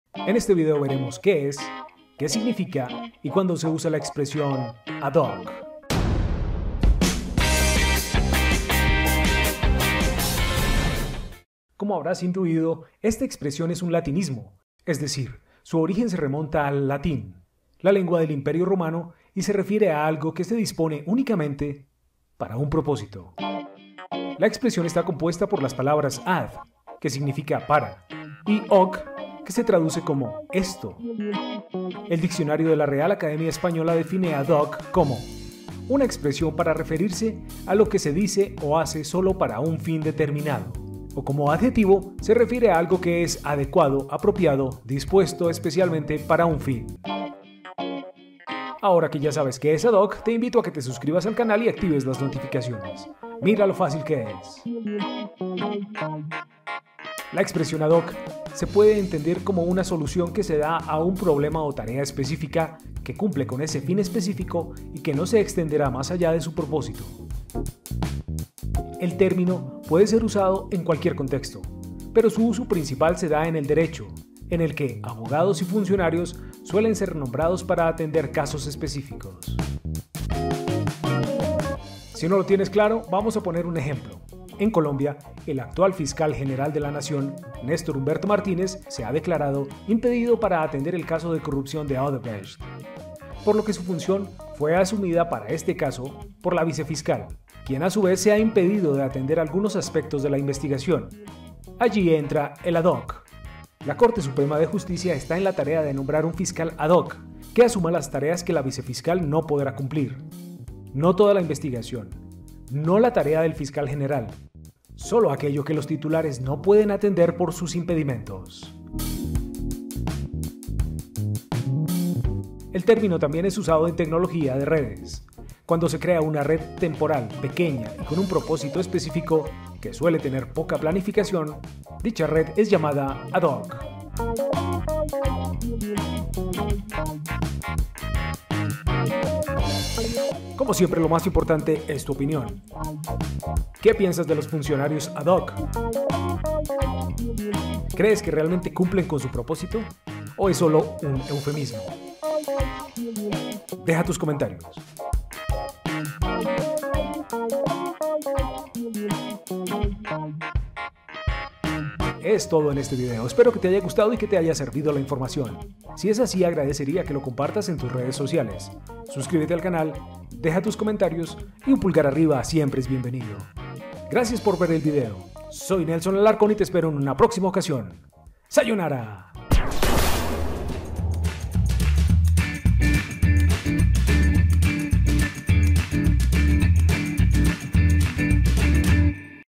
¿Qué significa ad hoc? 👨‍⚖️❓
En este video veremos qué es y qué significa y cuándo se usa la expresión "ad hoc".